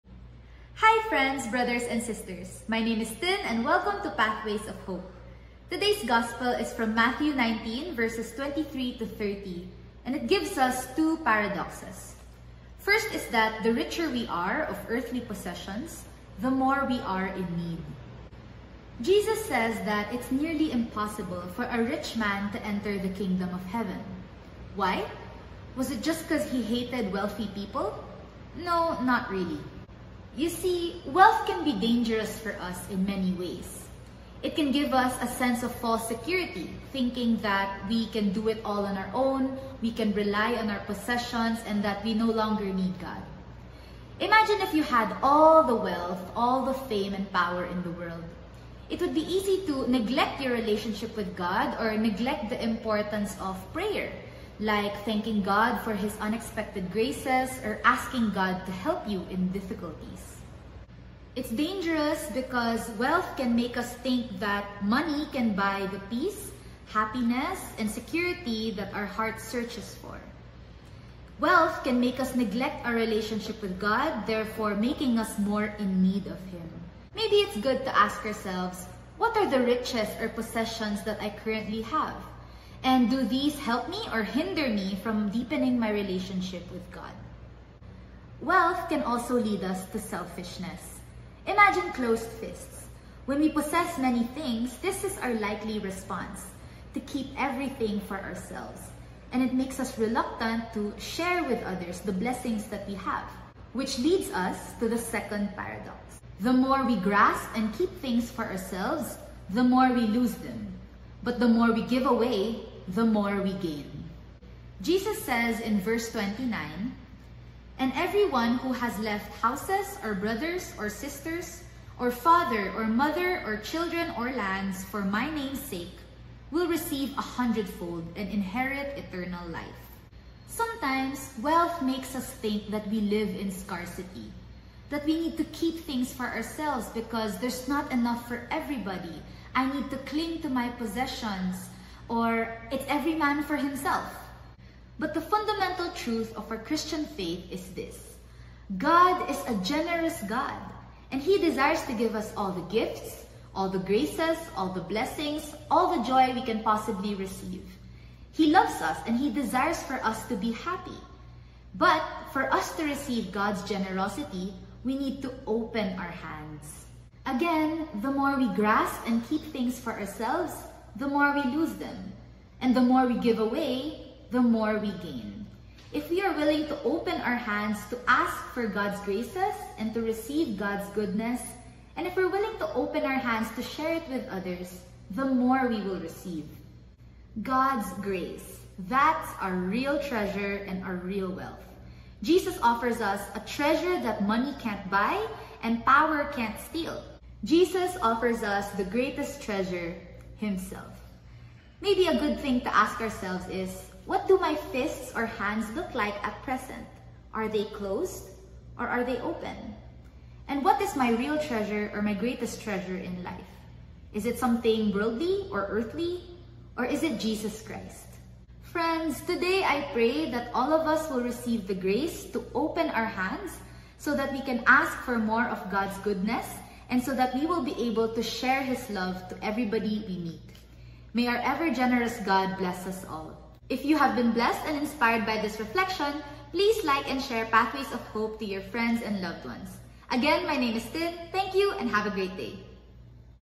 PATHWAYS OF HOPE : “The Difficulty of Wealth”
Tuesday of the 20th Week  in Ordinary Time
Memorial of The Queenship of the Blessed Virgin Mary

[GOSPEL OF THE DAY] :   MATTHEW 19: 23 - 30

Jesus said to his disciples:
"Amen, I say to you, it will be hard for one who is rich
to enter the Kingdom of heaven.
Again I say to you,
it is easier for a camel to pass through the eye of a needle
than for one who is rich to enter the Kingdom of God."
When the disciples heard this, they were greatly astonished and said,
"Who then can be saved?"
Jesus looked at them and said,
"For men this is impossible,
but for God all things are possible."
Then Peter said to him in reply,
"We have given up everything and followed you.
What will there be for us?"
Jesus said to them, "Amen, I say to you
that you who have followed me, in the new age,
when the Son of Man is seated on his throne of glory,
will yourselves sit on twelve thrones,
judging the twelve tribes of Israel.
And everyone who has given up houses or brothers or sisters
or father or mother or children or lands
for the sake of my name will receive a hundred times more,
and will inherit eternal life.
But many who are first will be last, and the last will be first."

Video by Tin Yu: Licensed Physical Therapist. Full-time missionary— Christ’s Youth in Action. Underway member— Ang Ligaya ng Panginoon Community.